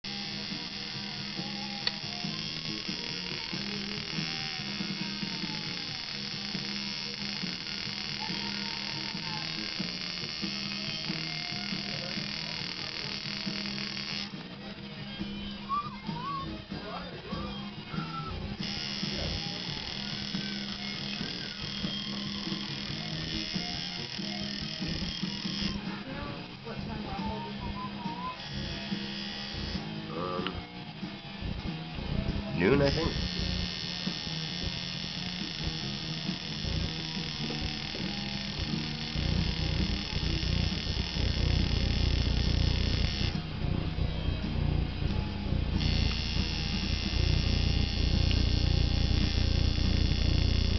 MY TATTOO GETTING TOUCHED UP
ME, GETTING THE TATS TOUCHED UP, FUCKING CAMERA DIES .BULLSHIT